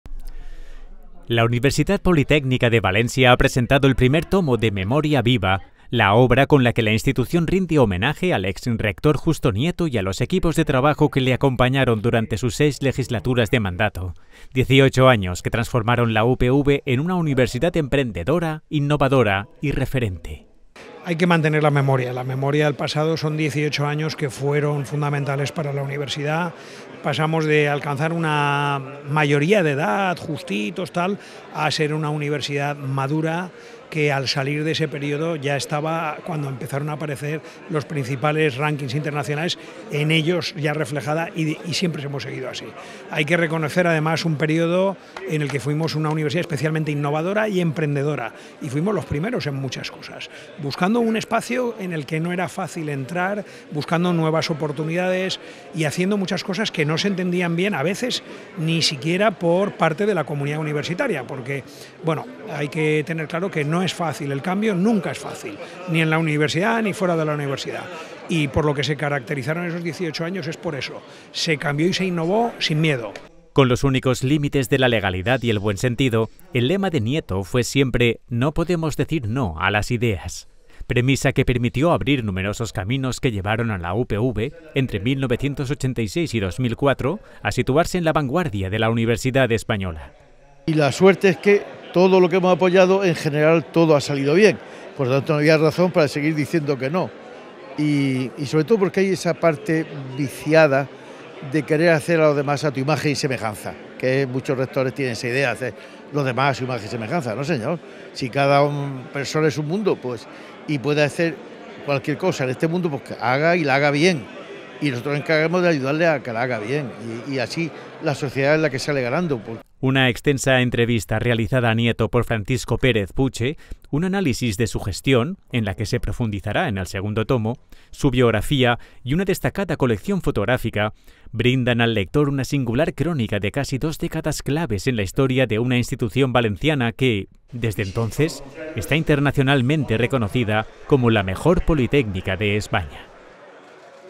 Memoria viva - Noticia @UPVTV; 03-06-2024
La Universitat Politècnica de València ha presentado el primer tomo de “Memoria viva”, la obra con la que la institución rinde homenaje al ex rector Justo Nieto y a los equipos de trabajo que le acompañaron durante sus 6 legislaturas de mandato, 18 años que transformaron la UPV en una universidad emprendedora, innovadora y referente.

Coordinado por José Jaime Gómez Hernández, el proyecto cuenta con un primer tomo que “enfatiza el papel crucial de Justo Nieto como rector” y viene prologado por el actual máximo mandatario de la UPV, José E. Capilla, “figura indispensable para la gestación de esta obra” en palabras de Gómez, y a quien Nieto, tal y como él mismo indica, está “especialmente agradecido por su voluntad de dignificar el pasado”.